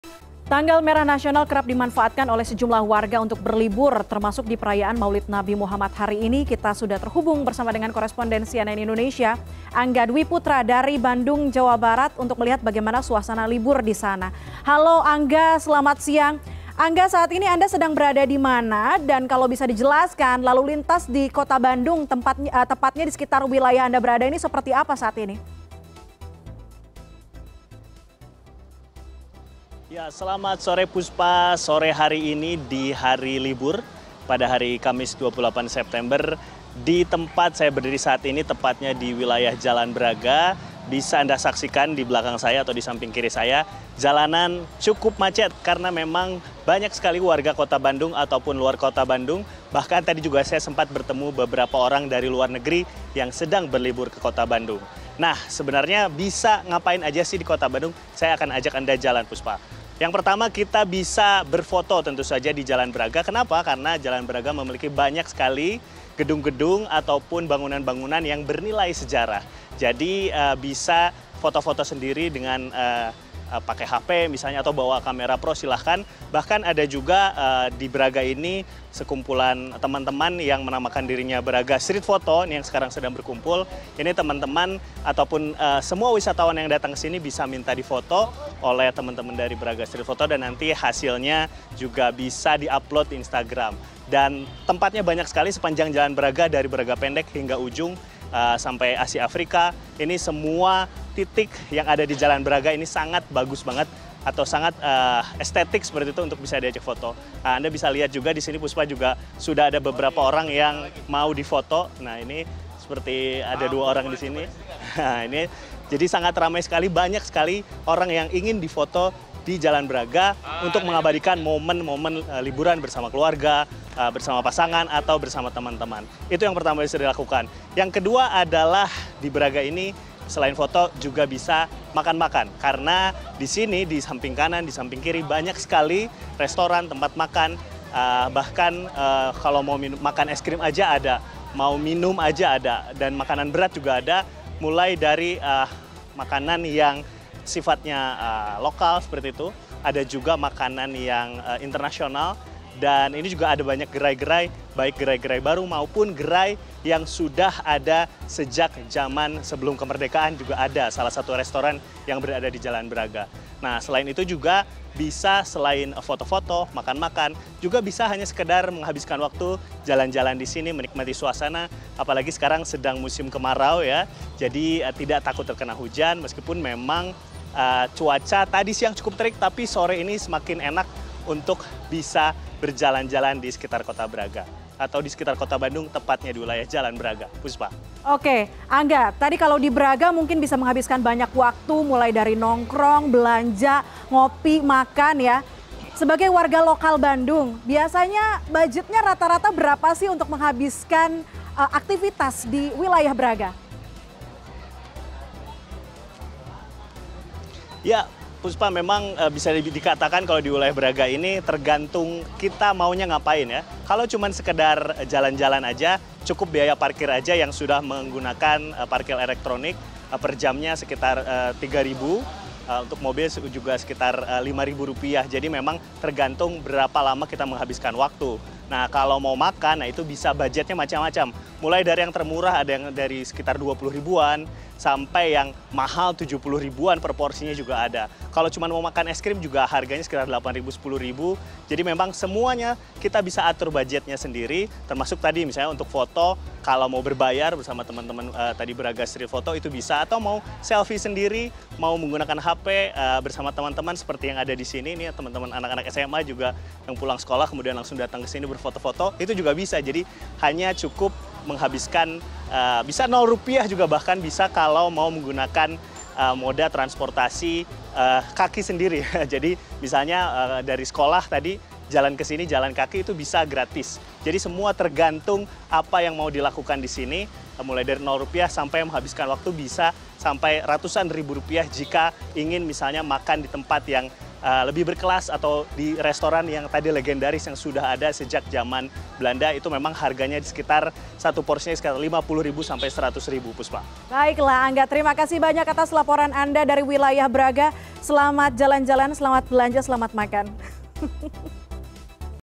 Libur Maulid Nabi, Kawasan Braga Dipadati Warga
Tanggal merah nasional kerap dimanfaatkan oleh warga untuk berlibur, termasuk di perayaan Maulid Nabi Muhammad SAW hari ini. CNN Indonesia Connected sudah terhubung dengan Koresponden CNN Indonesia, Angga Dwi Putra, dari kawasan Bagra, Bandung, Jawa Barat. Seperti apa suasana libur Maulid Nabi di Kota Kembang?